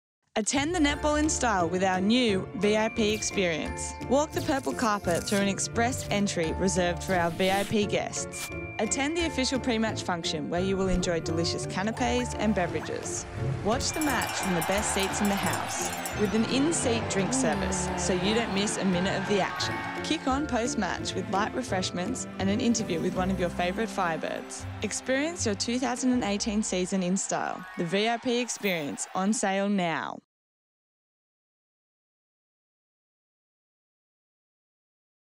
NEW VIP Experience
Experience your 2018 season in style with our NEW VIP Experience! Attend the official Firebirds pre-match and post-match functions where you will fill up on delicious canapes and beverages, watch the match from your reserved platinum seat with an in-seat drinks service so you don't miss a minute of the action. Purchase your tickets now